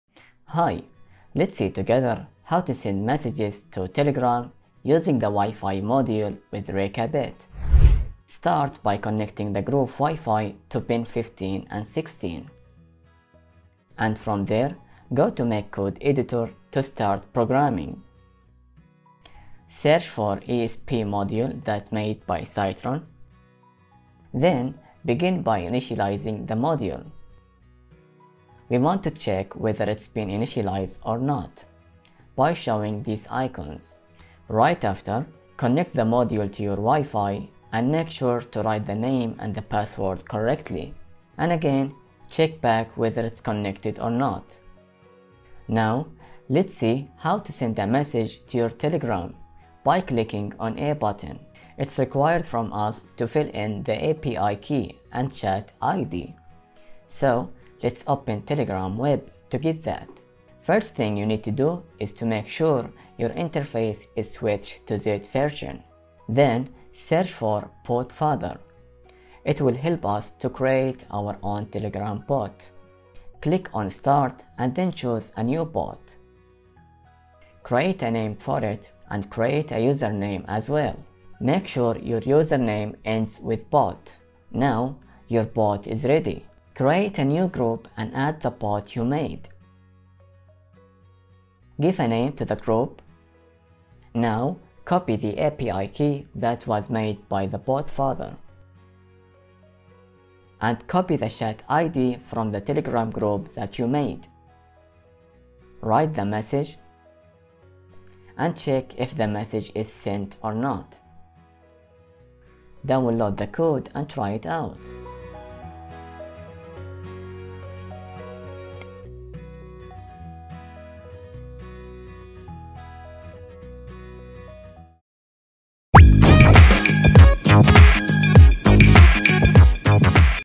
How to Send Telegram Messages Using Micro:bit and Grove WiFi 8266 Module
Send messages to Telegram using Micro:bit and the Grove WiFi 8266 module! In this video, you’ll learn how to initialize the WiFi module and create your own Telegram bot step by step. This powerful feature unlocks endless possibilities for IoT projects that send notifications via Telegram, making it a perfect addition to your creative ideas.